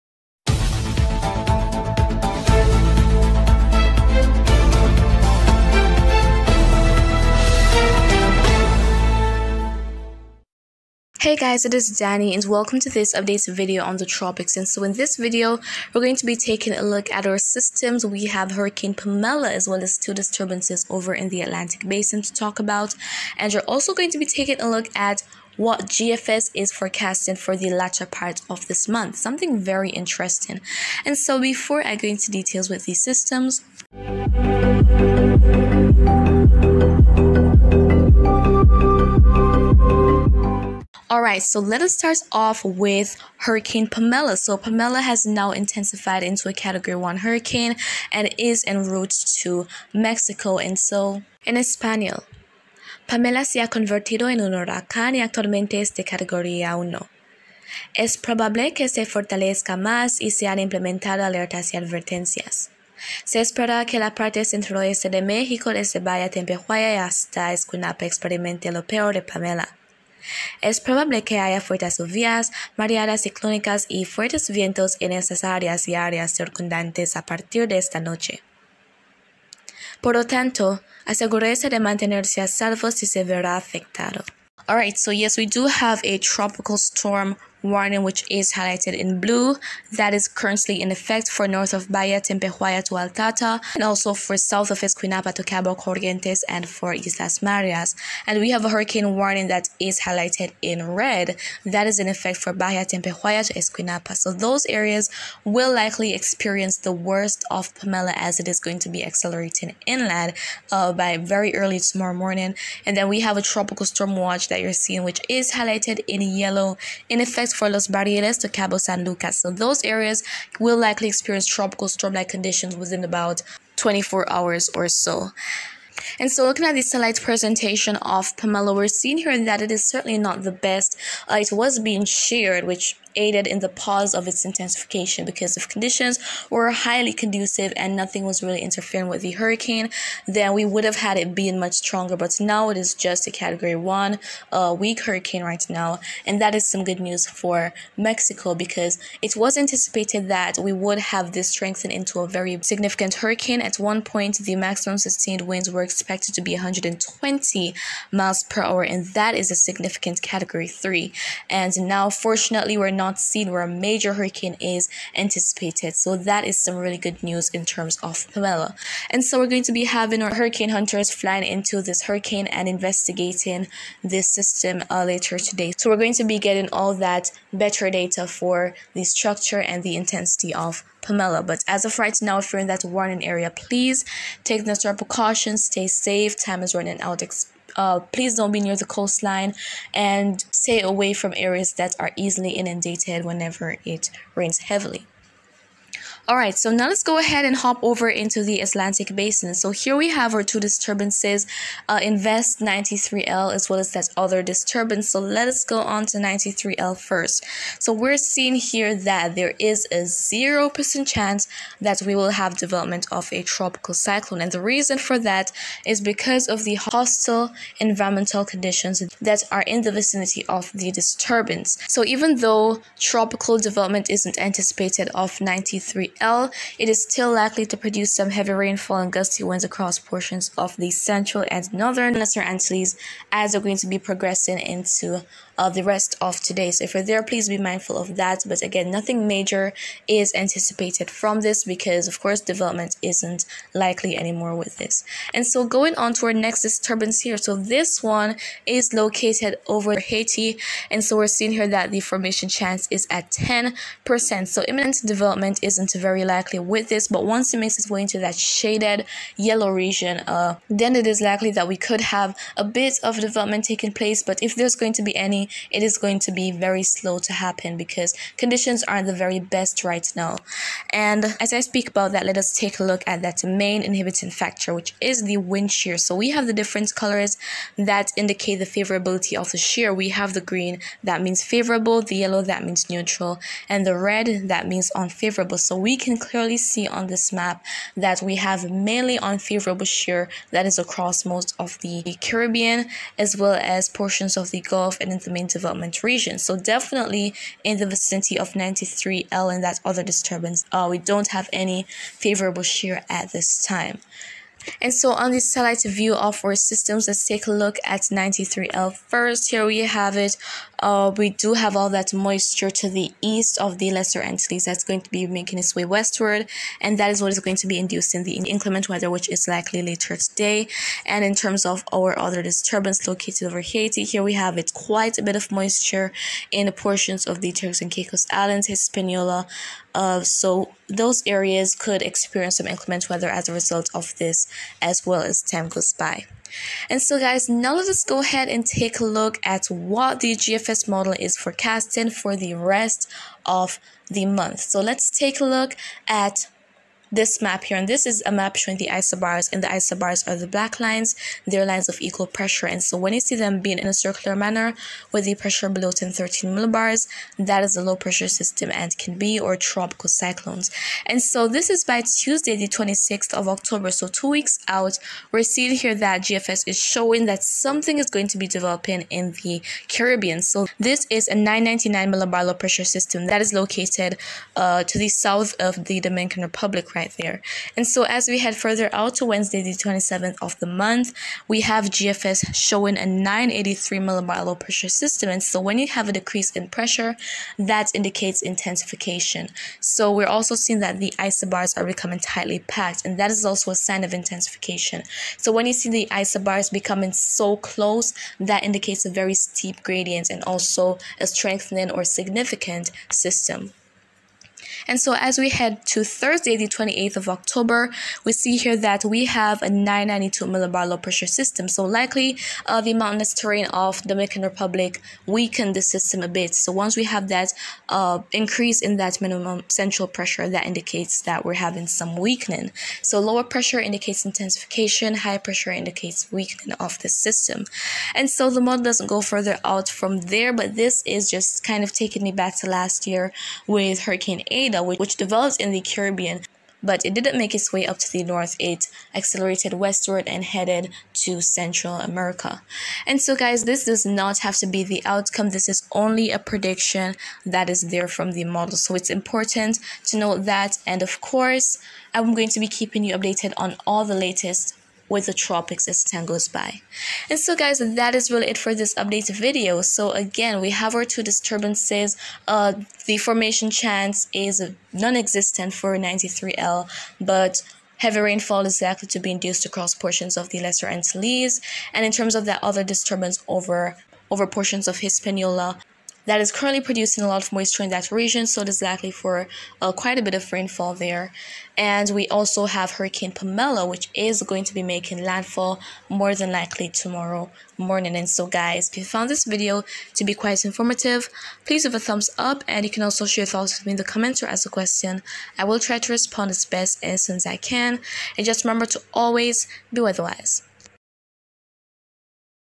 Invest 93L to bring heavy rainfall to Lesser Antilles • Second disturbance & PAMELA updates
Hey everyone, this update covers the latest on the disturbances in the Atlantic as well as Hurricane Pamela in the Eastern Pacific which will make landfall in Mexico.

~weather.gurl.danii
~daniihenryofficial